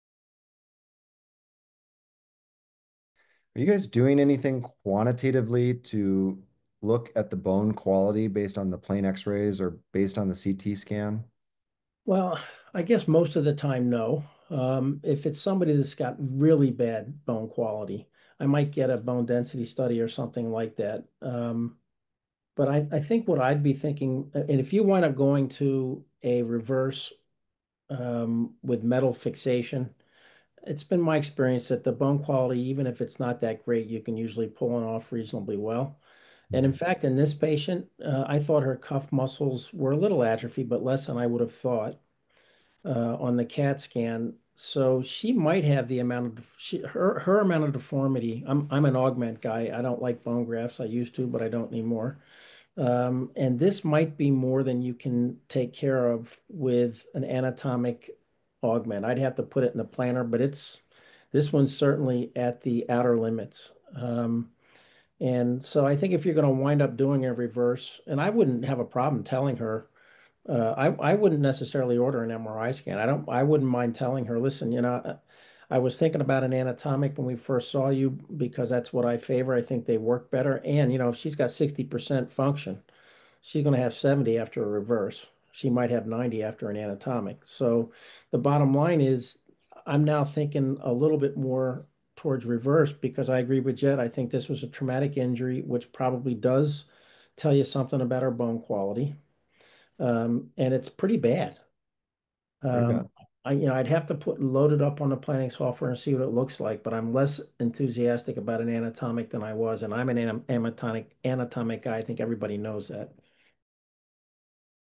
Bone Quality and Implant Choice in Shoulder Surgery
Here is a clip from our latest episode of CoinFlips & Controversies, “Progressive Shoulder Pain in 67F” featuring expert faculty from the Mid Atlantic Shoulder & Elbow Society Annual Meeting (taking place September 5, 2025 at the Ritz-Carlton in Washington, DC.)

Watch Drs. Knudsen & Williams, discuss “Bone Quality and Implant Choice in Shoulder Surgery”.